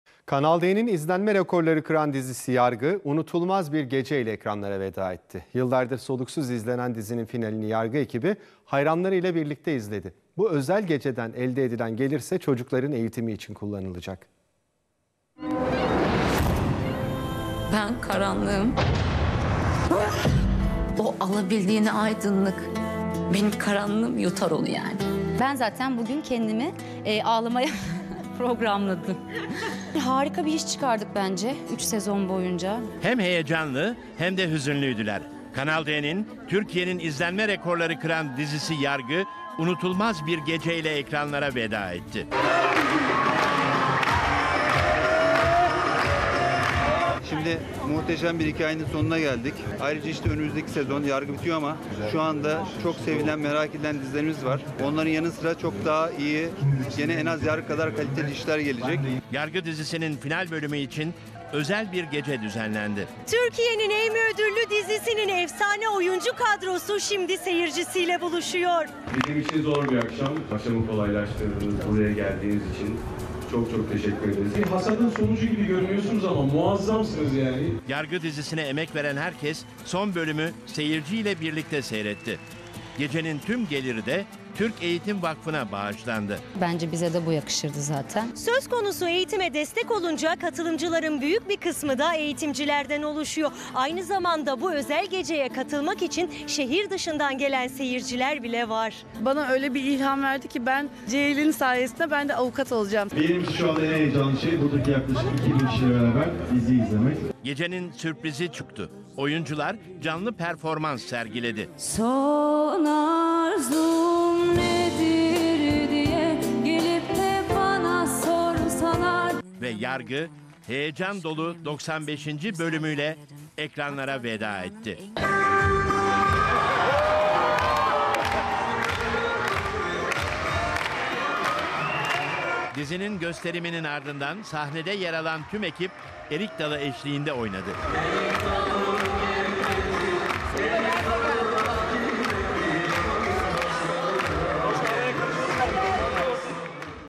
Reyting Rekortmeni Yargı Dizisi Muhteşem Yardım Gecesiyle Ekranlara Veda Etti!
Reyting rekortmeni Yargı dizisi muhteşem yardım gecesiyle ekranlara veda etti. Yardım gecesinde Yargı ekibi, final bölümü seyirciyle birlikte izledi...